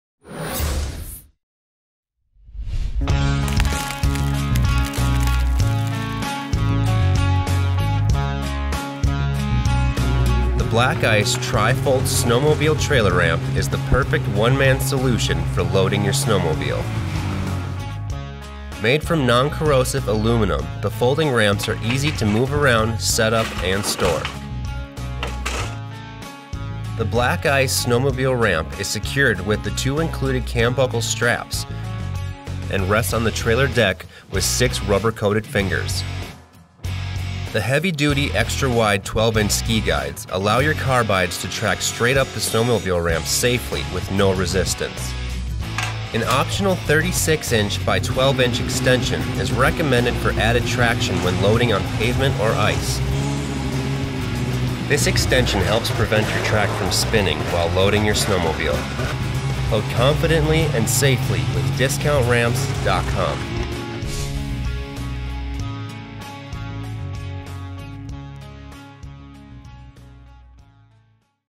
Black Ice Tri-Fold Snowmobile Trailer Ramps
In this video we cover the features of the Black Ice Tri-Fold Snowmobile Trailer Ramps. Available with or without an extension for the center rungs, the ramps feature three sections specifically designed to grip and guide your snowmobile directly into your trailer quickly and safely, including extra-wide polyurethane ski guides for low-resistance loading.